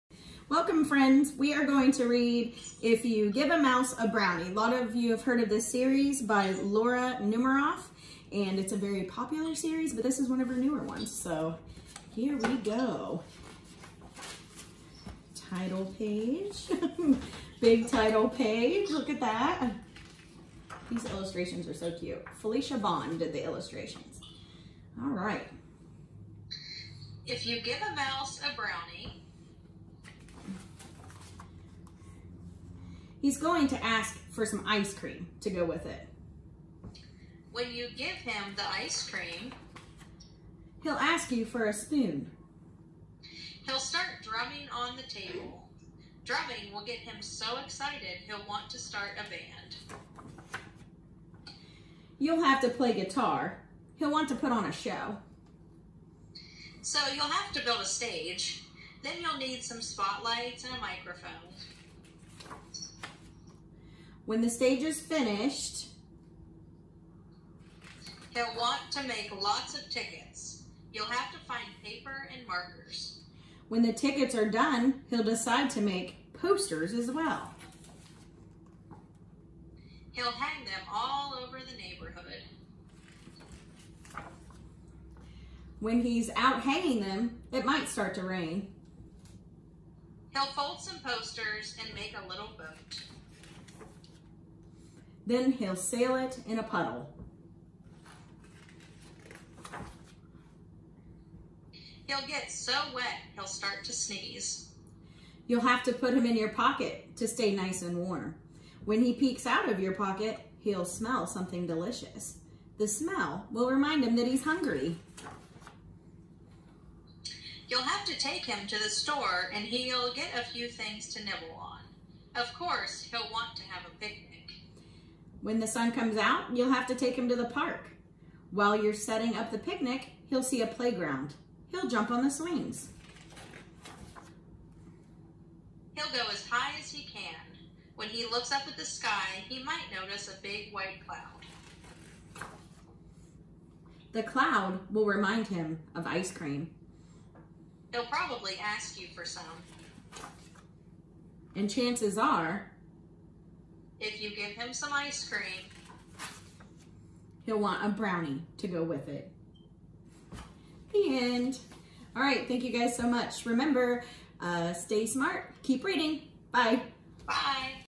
If You Give a Mouse a Brownie
Mrs. Stull and Miss Merriman read the sequel to the classic... If You Give a Mouse a Cookie! We love this author and illustrator!